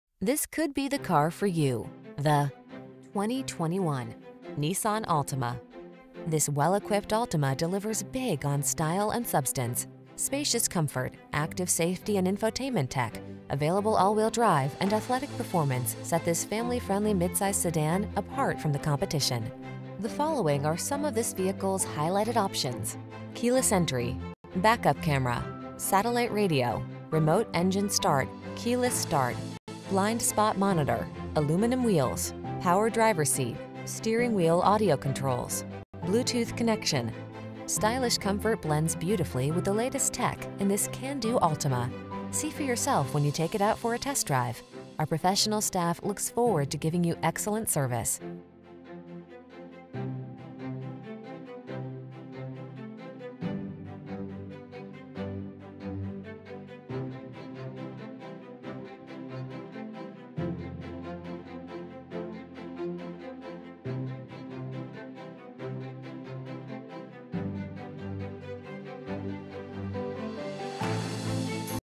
2021 Nissan Altima OakRidge, Knoxville, Maryville, Lenoir City, Clinton, TN 33402N
Storm Blue Metallic New 2021 Nissan Altima available in Oak Ridge, Tennessee at Oak Ridge Nissan. Servicing the Knoxville, Maryville, Lenoir City, Clinton, TN area.
2021 Nissan Altima 2.5 SV - Stock#: 33402N - VIN#: 1N4BL4DV6MN345092